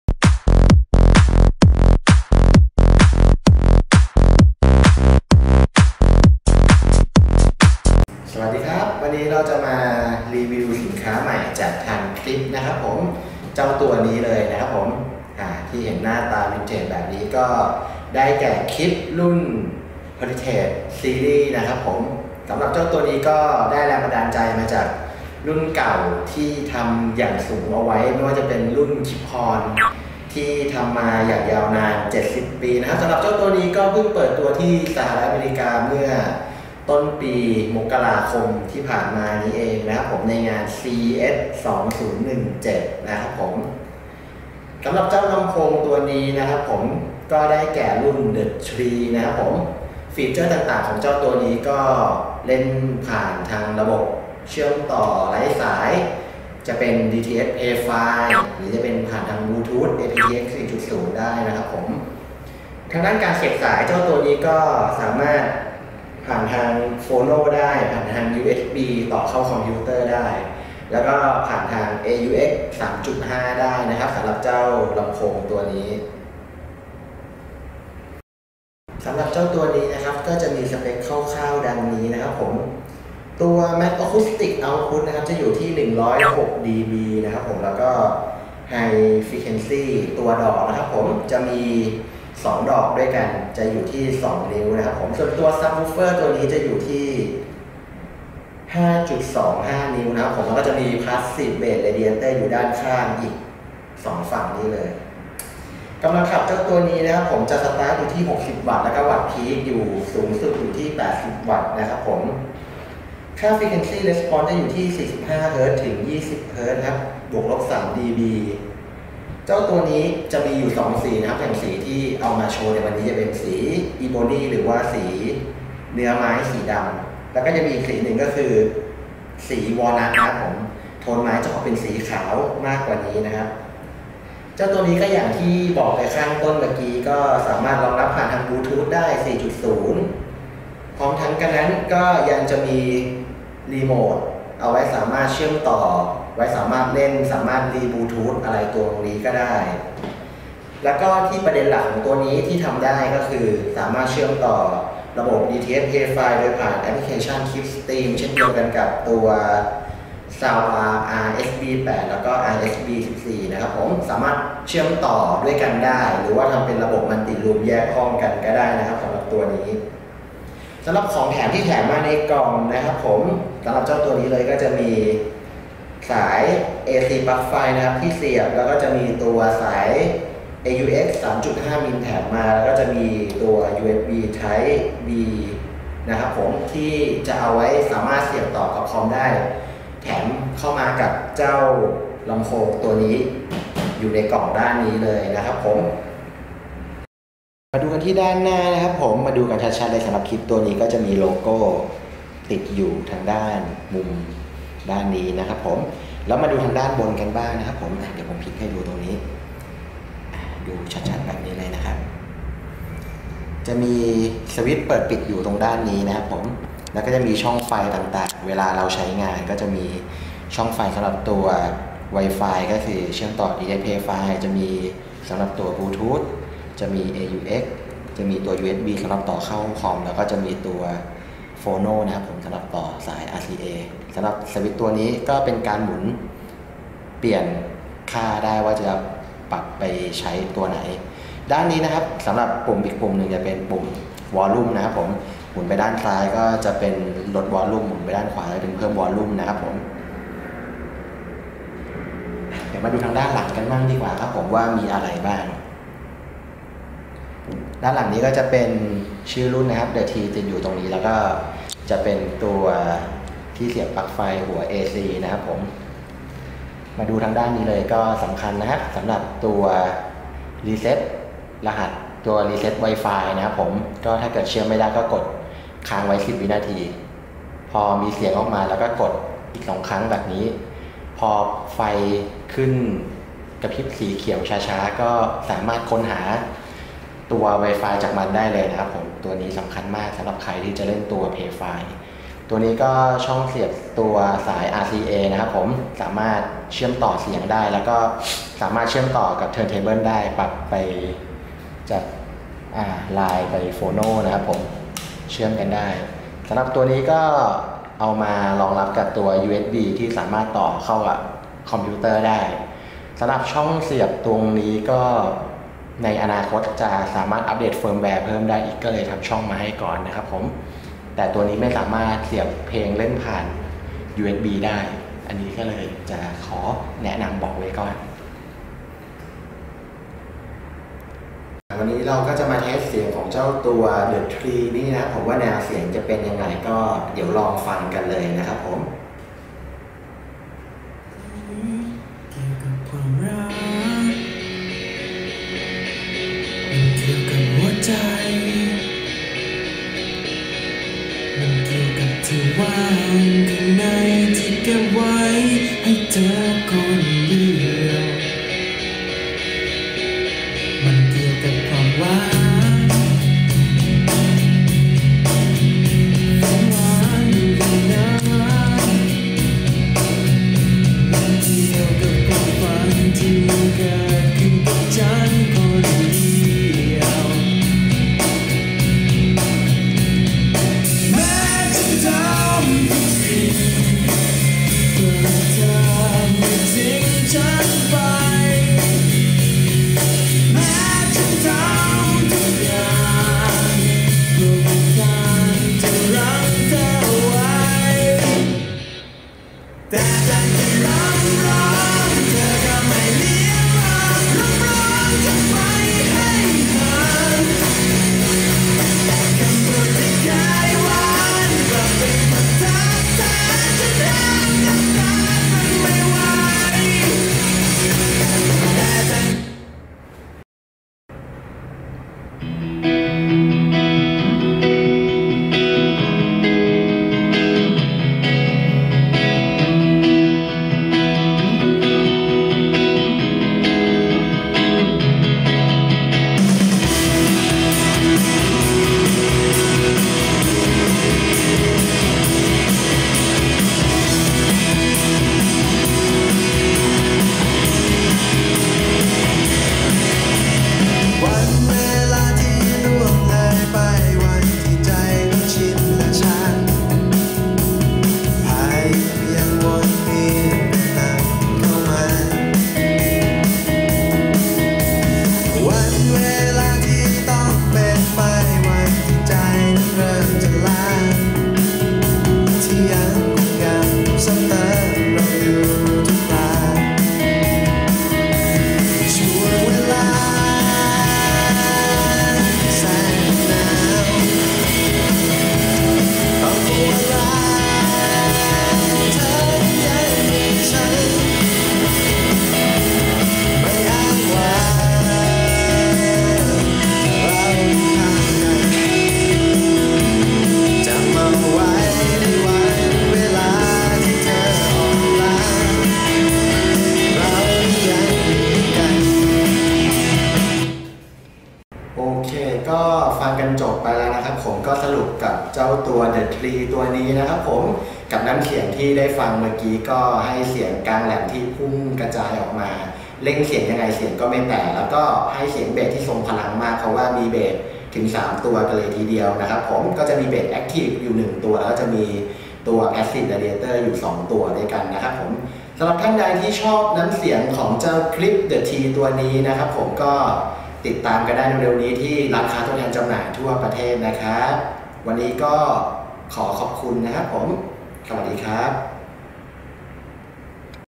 !!! Review: Klipsch THE THREE @SoundRepublic !!!
Klipsch "THE THREE"
1 ใน ลำโพงชุด Heritage Wireless ชื่อรุ่น THE THREE
เป็นลำโพงแนววินเทจได้รับแรงบันดาลใจมากจากรุ่นใหญ่อย่าง Heritage series โดยรุ่นนี้จะเป็นลำโพงระบบ Active มีกำลังขับสูงสุดที่ 80 watts มีดอก ที่ให้เสียงกลาง HIGH FREQUENCY DRIVERS 2 x 2"1/4 และ เบส subwoofer 5.25” x 1 ดอก พร้อม passive radiators 5.25” x 2 ดอกเลยทีเดียว เลยมั่นใจได้ว่ารุ่นนี้นั้นให้เสียงเบสที่เต็มอิ่มจุใจแน่นอน สำหรับลำโพงรุ่นนี้นั้นสามารถเชื่อมต่อได้ทั้ง Bluetooth , Aux3.5 , USB Type B สำหรับคอมพิวเตอร์และ notebook , Phono pre-amp / RCA analog ที่สามารถสับสวิต์มาเป็น phono - Line เพื่อต่อกับเครื่องเล่น turn table ได้อีกด้วย และ ที่สำคัญสามารถรองรับ DTS PLAY-FI ผ่านการเล่น Appication klipsch streem อีกด้วย